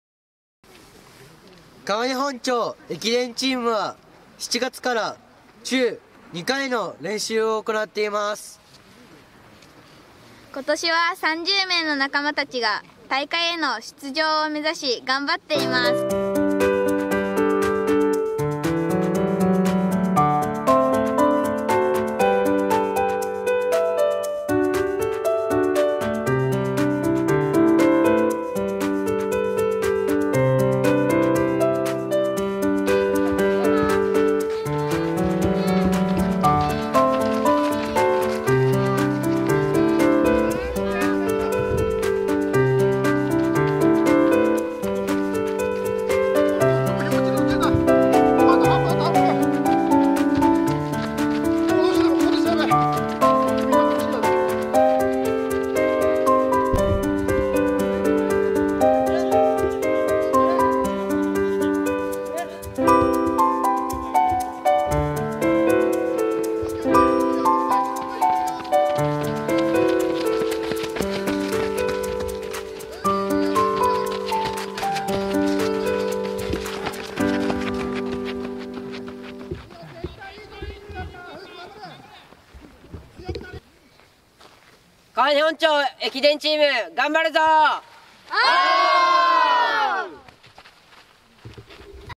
応援動画〜川根本町〜【しずおか市町対抗駅伝2022】
「しずおか市町対抗駅伝」は皆様に支えられ今年、23回を迎えることができました。
このしずおか市町対抗駅伝がふるさとを元気にする力になるよう、私たちも準備を進めます。
県内各市町が特色を活かした応援動画で、ふるさとの誇りを胸に出場する選手を応援します。

つなごう。ふるさとへの思いを。未来への希望を。